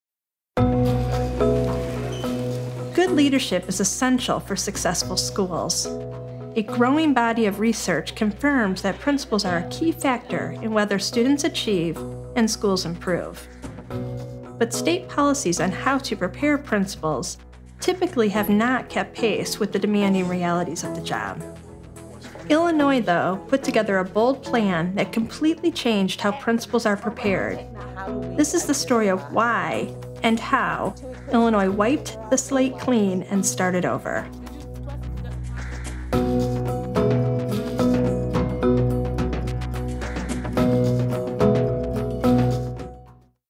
A Bold Move to Better Prepare Principals: Trailer
This trailer introduces A Bold Move to Better Prepare Principals: The Illinois Story, a four-part video series showing how one state worked with universities, nonprofits and districts to change the way principals are trained and licensed, so they become more effective at  improving classroom instruction.

The series is part of a major effort at Wallace to improve university preparation programs for principals. Read more about the $47-million initiative to develop models for improving university programs and broadly share insights here: